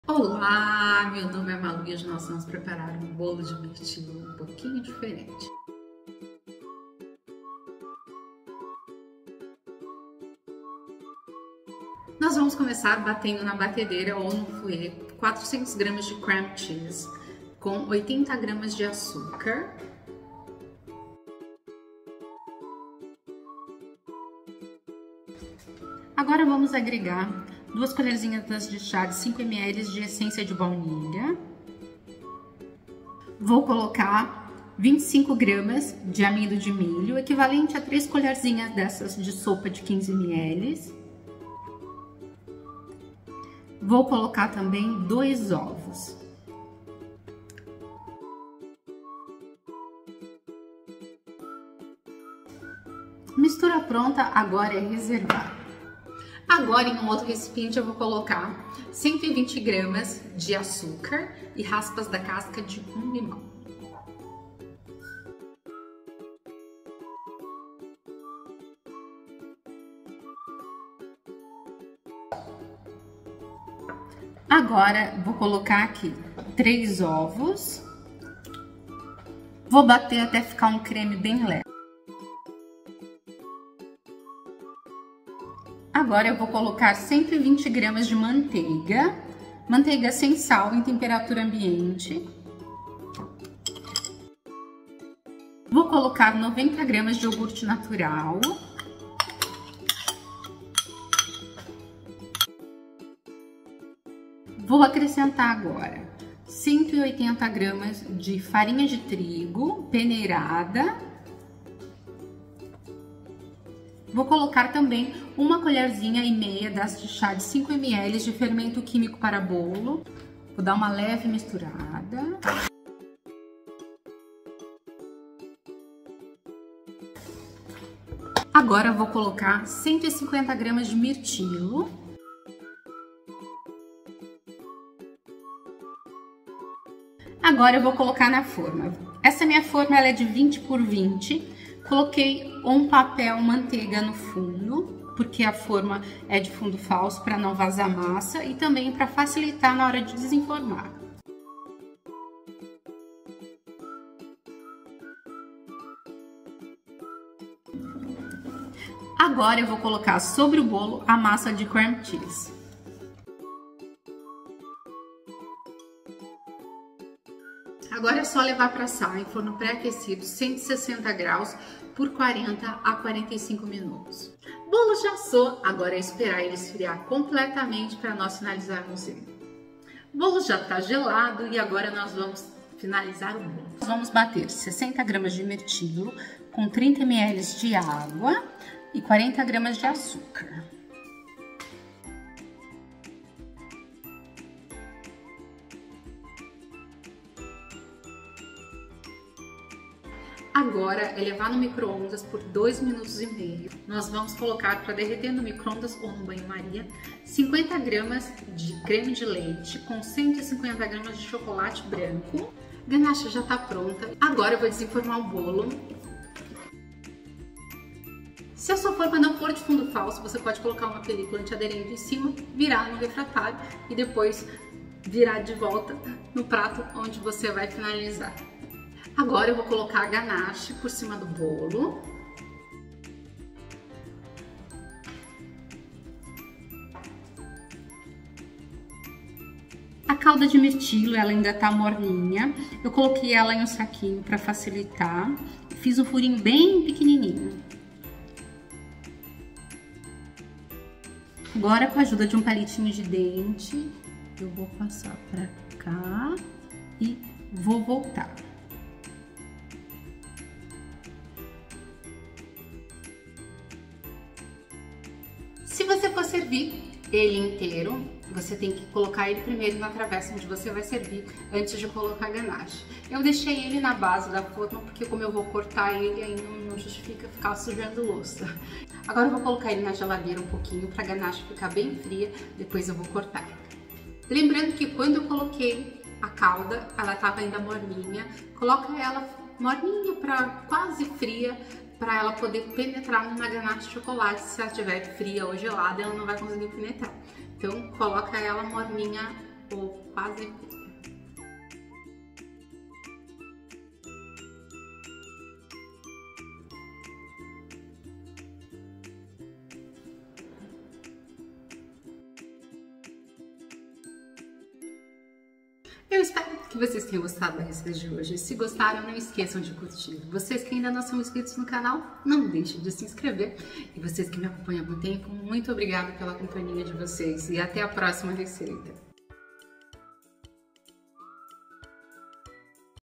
Bolo de mirtilo com limão - Uma sobremesa perfeita para qualquer ocasião.
O bolo de mirtilo com limão e cream cheese é uma explosão de sabores frescos e cremosos. Cada mordida revela a doçura dos mirtilos, o toque cítrico do limão e a suavidade irresistível do cream cheese, resultando em uma sobremesa perfeita para qualquer ocasião.

**Ingredientes:**
- 3 ovos
- 120 gramas de açúcar
- 1 colher de chá de essência de baunilha (5 ml)
- 85 gramas de iogurte natural sem açúcar
- Raspas da casca de um limão
- 120 gramas de manteiga
- 180 gramas de farinha de trigo
- 1 ½ colher de chá (5 ml ) de fermento químico para bolo
- 150 gramas de mirtilos

**Para a Massa de Queijo (Cream Cheese):**
- 300 gramas de cream cheese
- 80 gramas de açúcar
- 1 ovo
- 1 colher de chá de essência de baunilha (5 ml)
- 25 gramas de amido de milho (equivalente a 3 colheres de sopa de 15 ml)

**Para a Cobertura (Ganache de Chocolate Branco):**
- 150 gramas de chocolate branco
- 50 gramas de creme de leite

**Para o calda de Mirtilo:**
- 30 ml de água
- 40 gramas de açúcar
- 60 gramas de mirtilo

**Instruções:**
1. Comece miaturando o açúcar às raspas de limão, adicione ovos, obter até ficar uma mistura homogênea. Adicione a manteiga em temperaturaambiente, o iogurte e a baunilha, misturando bem.

2. Em seguida, adicione o fermento químico e a farinha de trigo, misturando até incorporar. Acrescente os mirtilos à massa e mexa delicadamente.

3. Coloque a massa em uma forma de aro 20x20 cm, previamente untada com manteiga e forrada com papel manteiga.

4. Para a massa de queijo, bata o cream cheese com o açúcar, o ovo, a essência de baunilha e o amido de milho até obter um creme homogêneo. Espalhe essa mistura sobre a massa do bolo de limão.

5. Asse em forno pré-aquecido a 160 graus Celsius por 40 a 45 minutos.

6. Retire o bolo do forno e deixe esfriar completamente antes de aplicar a cobertura.

7. Para a cobertura, derreta o chocolate branco com o creme de leite e espalhe sobre o bolo já resfriado.

8. Prepare o suco de mirtilo combinando água, açúcar e mirtilos, peneire e leve ao micro-ondas por 2,5 a 3 minutos. Coloque o suco em um saquinho, faça listras sobre a cobertura e decore com um palito de dente para criar um padrão.

9. Desfrute do seu delicioso bolo de mirtilo com limão e cream cheese!